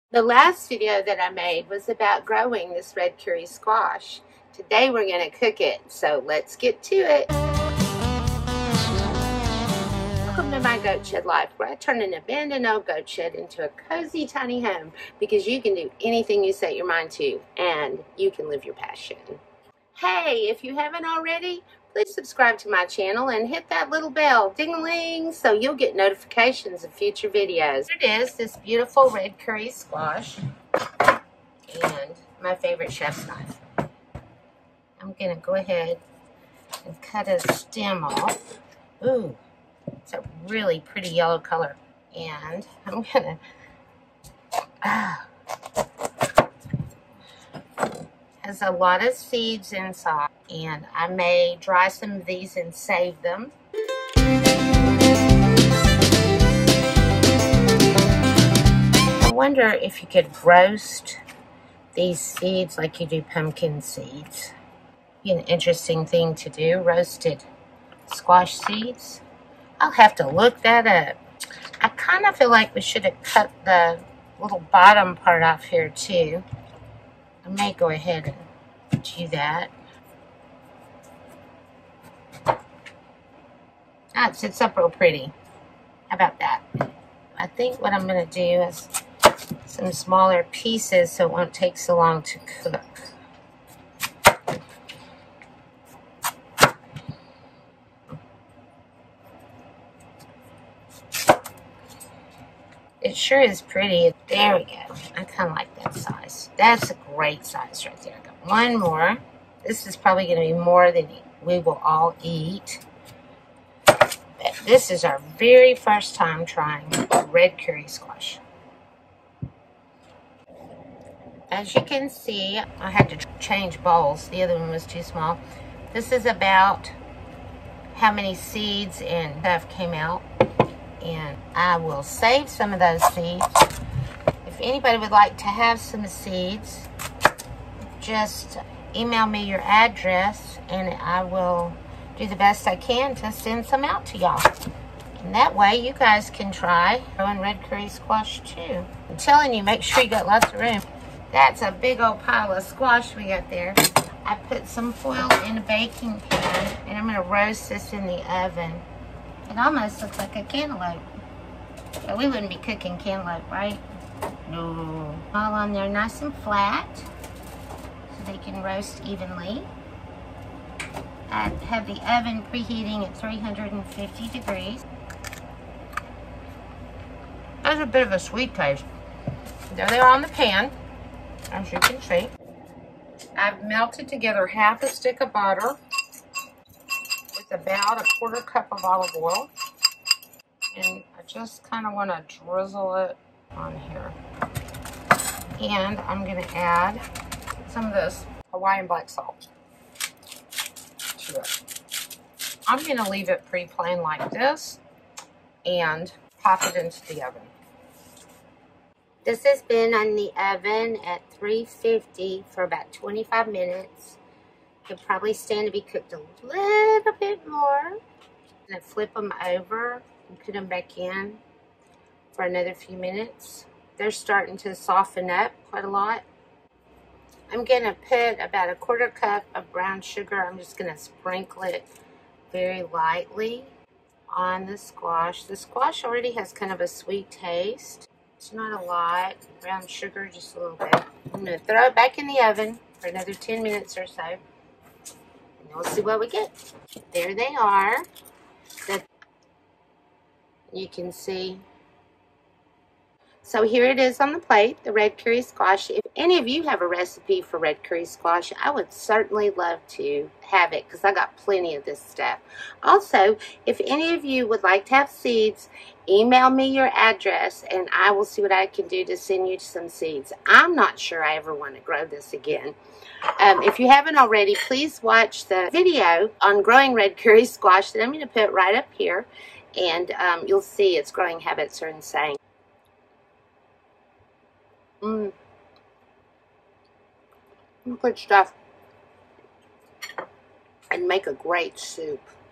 Cooking Red Kuri Squash
This video is about cooking Red Kuri Squash for the first time.  Never having cooked this type of squash before, I decided to use a really simple recipe.  I used butter, olive oil, salt and brown sugar and baked it for about 30-35 minutes.  As always thank you for visiting My Goat Shed Life and for cooking with me in my tiny kitchen.